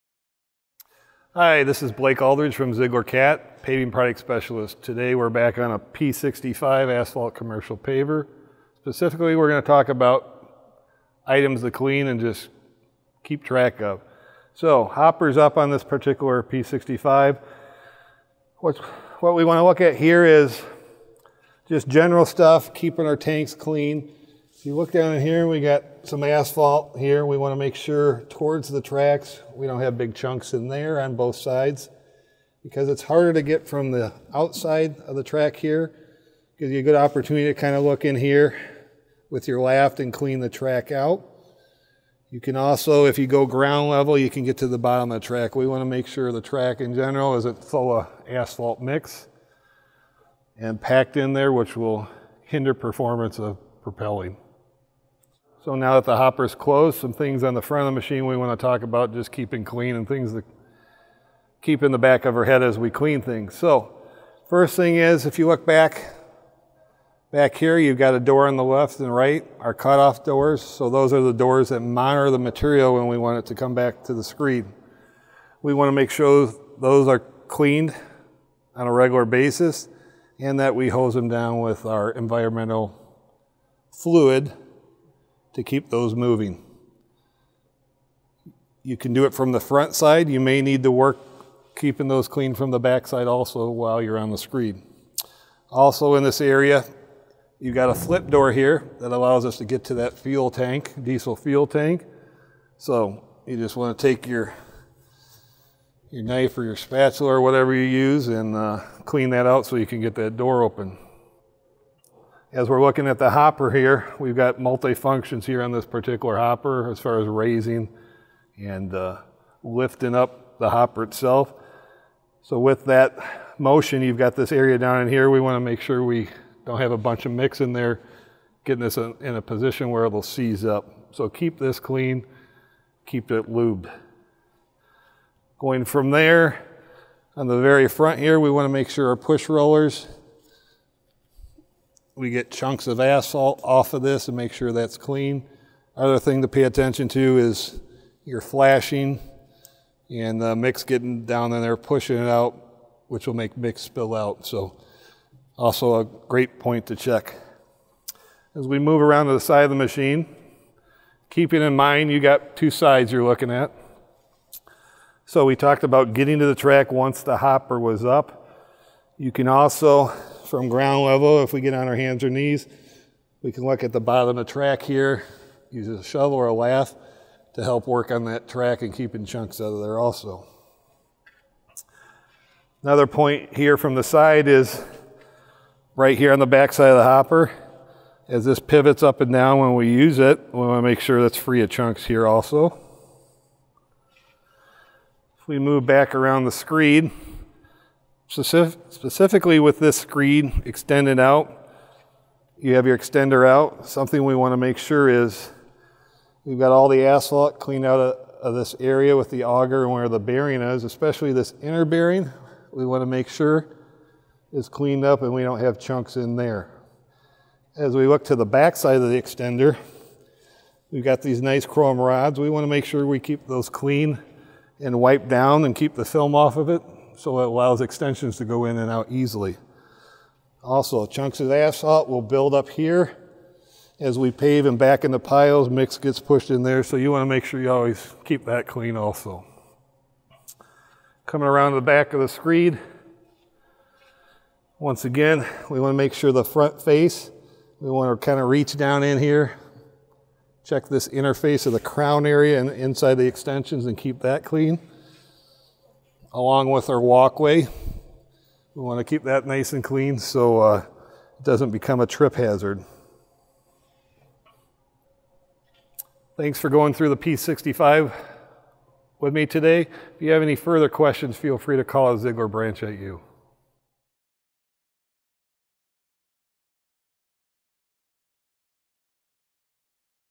Weiler P65 Commercial Paver - Cleaning Points
Learn the important areas to keep clean on the Weiler P65 Commercial Paver